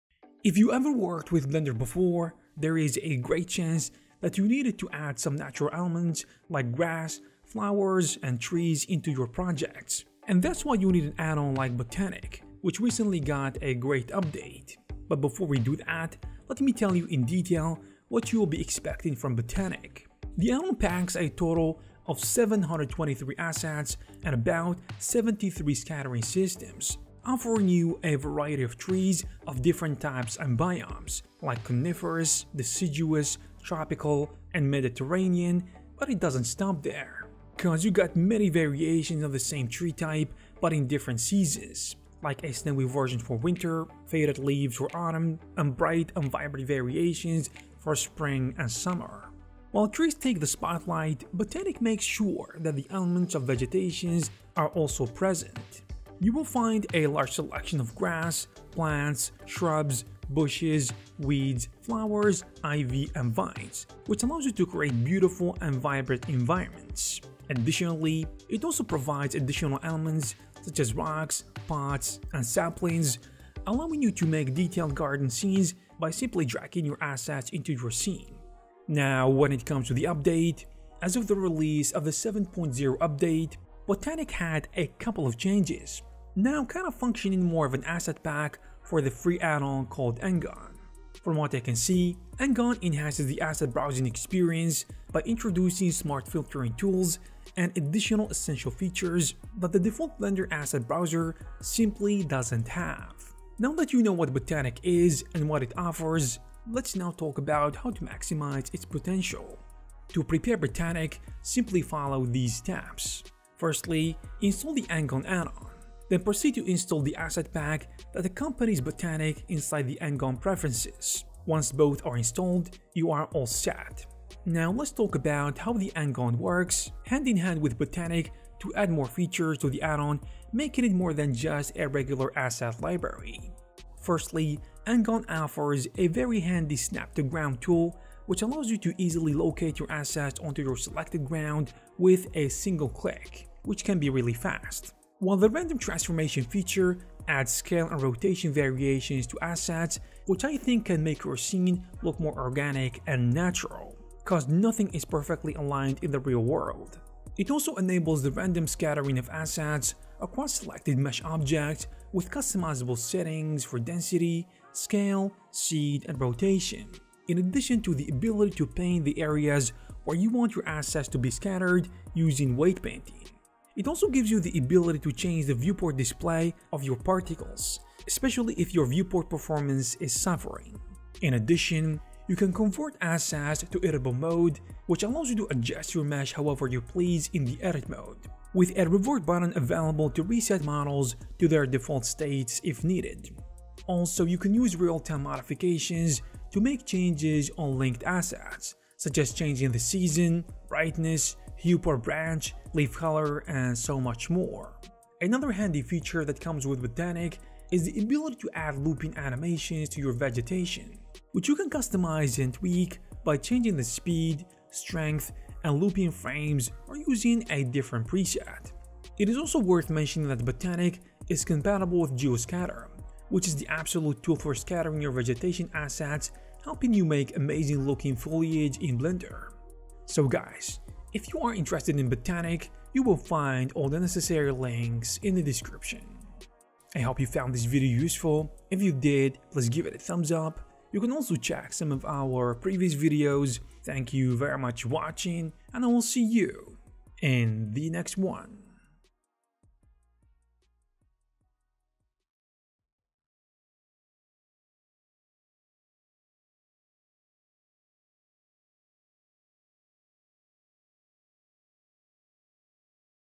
Vegetation Blender Addon Got An Update!
►Tree And Grass Library Botaniq - Trees

If you've ever worked with Blender, chances are you've needed to add some natural elements like grass, flowers, and trees into your projects.  that’s why you need an addon like Botaniq which recently got an update.